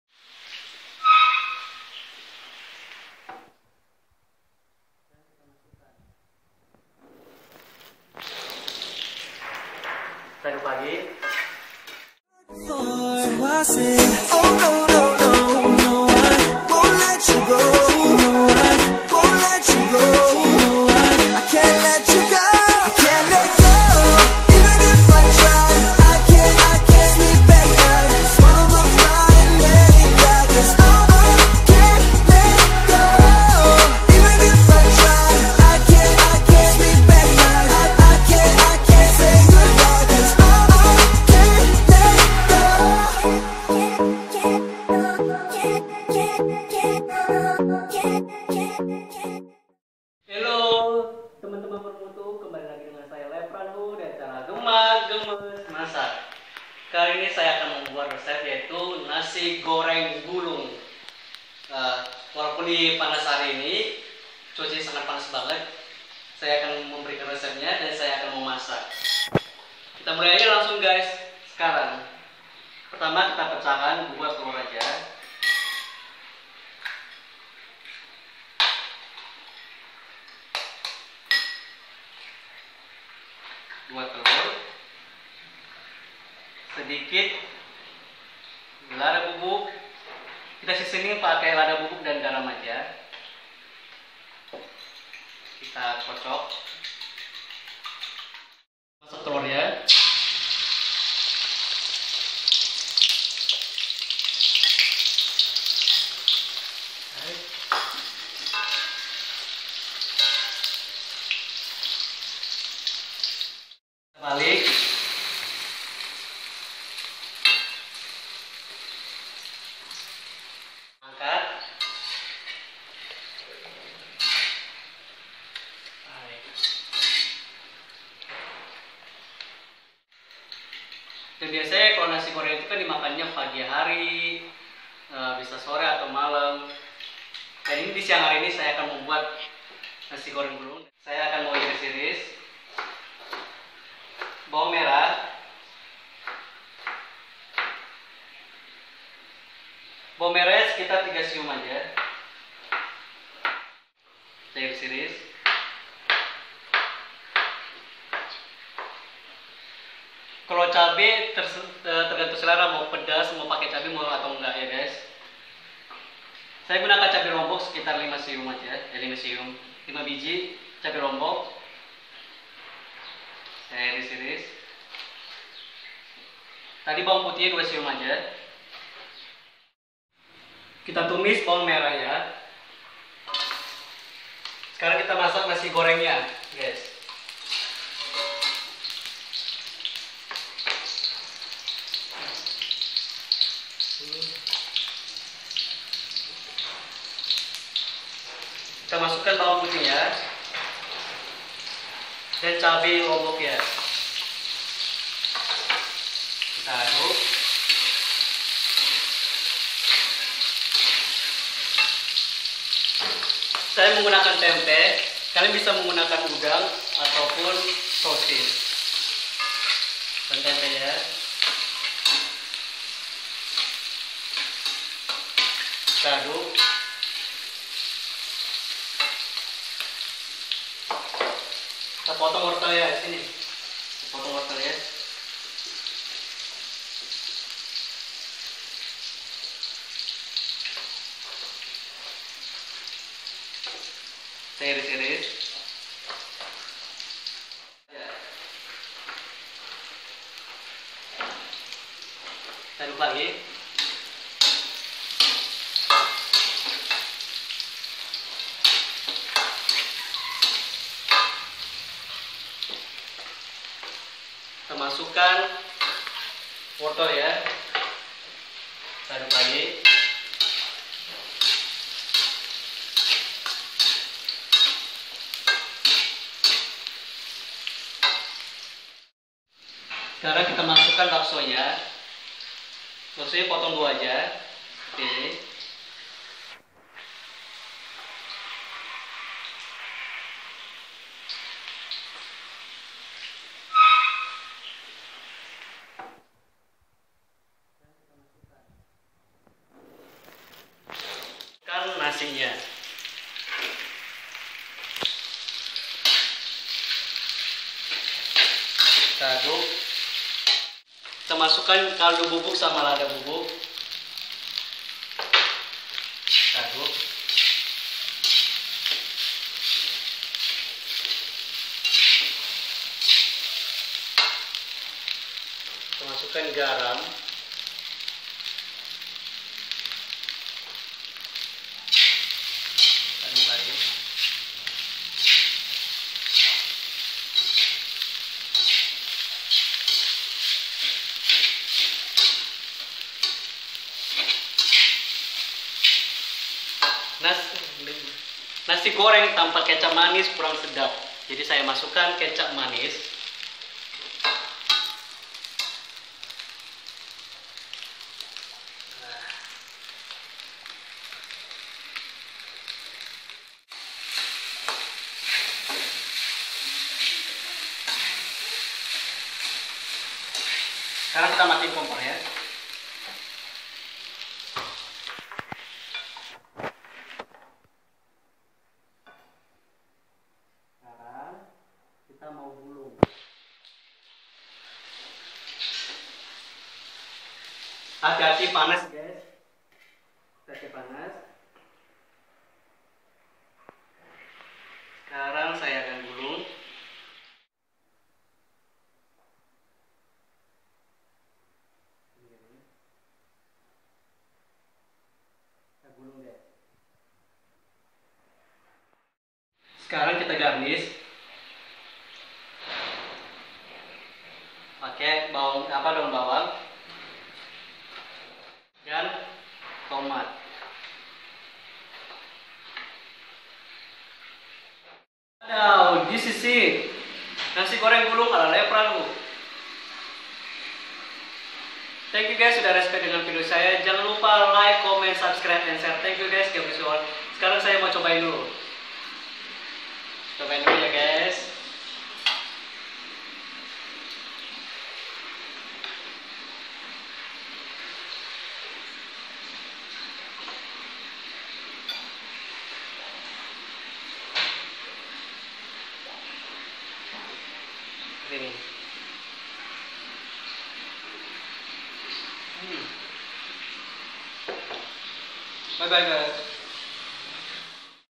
GEMAK ~ NASI GORENG GULUNG uhuyyy Enakkk Coeg | seperti Chef Farah Quinn dan Juna
" HIDUP LO HARUS SUSAH BIAR NGERASAIN SAKIT HATI AGAR LO BISA MENJADI PRIBADI LEBIH BAIK LAGI "..

BAHAN ;
NASI PUTIH
2 TELUR
TEMPE
WORTEL
BASO
BAWANG MERAH DAN GARLIC
5 CABE LOMBOK
KECAP MANIS
LADA DAN KALDU BUBUK
GARAM

Thanks Teman Bermutu..